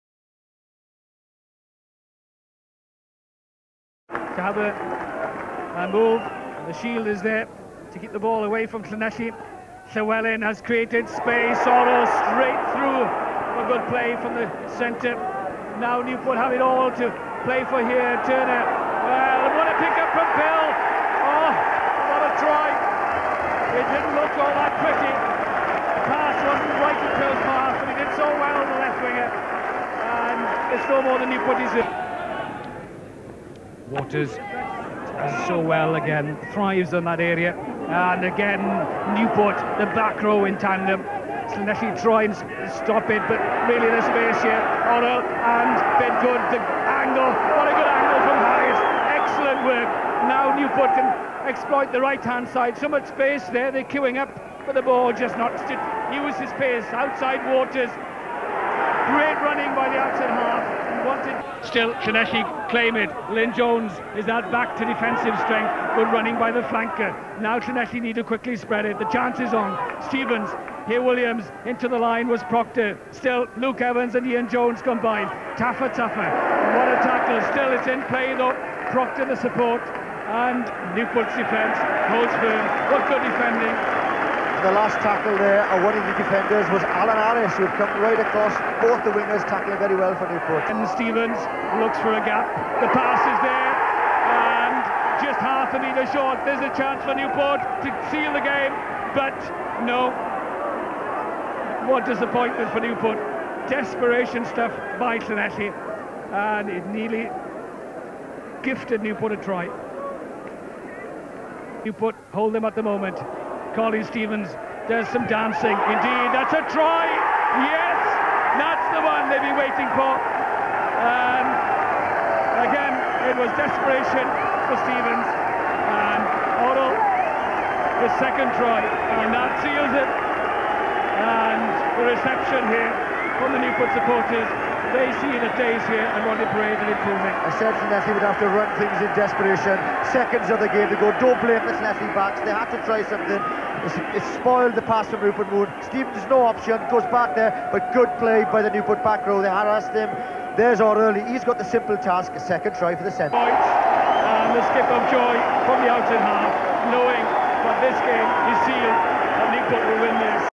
Newport v Llanelli
Second season of League rugby. Big crowd at Rodney Parade. Gloomy afternoon. West Walians beaten.